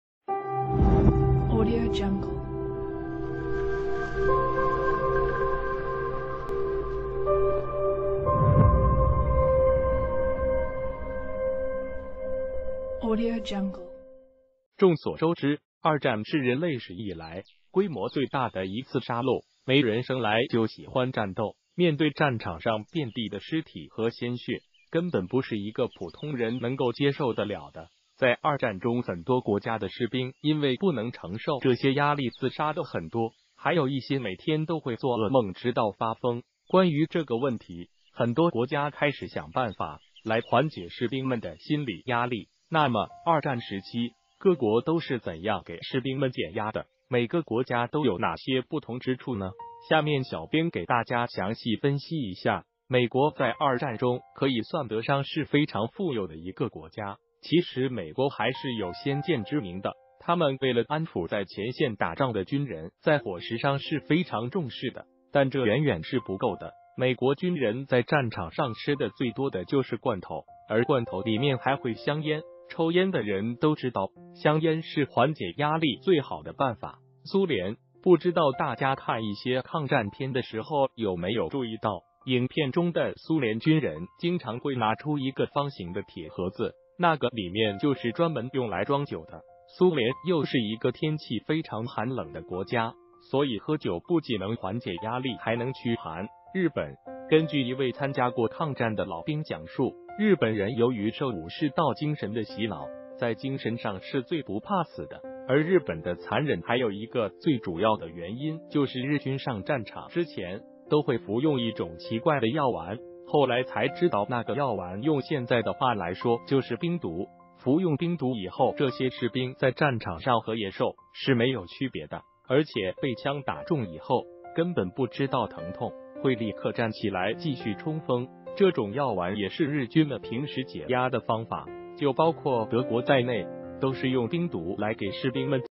二战各国是如何给士兵们减压的？这种办法只有日本人做得出来
谢谢！众所周知，二战是人类史以来规模最大的一次杀戮。没人生来就喜欢战斗，面对战场上遍地的尸体和鲜血根本不是一个普通人能够接受得了的。在二战中很多国家的士兵因为不能承受这些压力自杀的很多，还有一些每天都会做噩梦直到发疯。关于这个问题，很多国家开始想办法来缓解士兵们的心理压力.那么二战时期，各国都是怎样给士兵们减压的？每个国家都有哪些不同之处呢？下面小编给大家详细分析一下。美国在二战中可以算得上是非常富有的一个国家，其实美国还是有先见之明的.他们为了安抚在前线打仗的军人在伙食上是非常重视的，但这远远是不够的，美国军人在战场上吃的最多的就是罐头，而罐头里面还会香烟。抽烟的人都知道，香烟是缓解压力最好的办法。苏联，不知道大家看一些抗战片的时候有没有注意到，影片中的苏联军人经常会拿出一个方形的铁盒子，那个里面就是专门用来装酒的。苏联又是一个天气非常寒冷的国家，所以喝酒不仅能缓解压力还能驱寒。日本，根据一位参加过抗战的老兵讲述，日本人由于受武士道精神的洗脑，在精神上是最不怕死的。而日本的残忍还有一个最主要的原因就是日军上战场之前都会服用一种奇怪的药丸，后来才知道那个药丸用现在的话来说就是冰毒。服用冰毒以后这些士兵在战场上和野兽是没有区别的，而且被枪打中以后根本不知道疼痛，会立刻站起来继续冲锋。这种药丸也是日军们平时解压的方法，就包括德国在内都是用冰毒来给士兵们解压。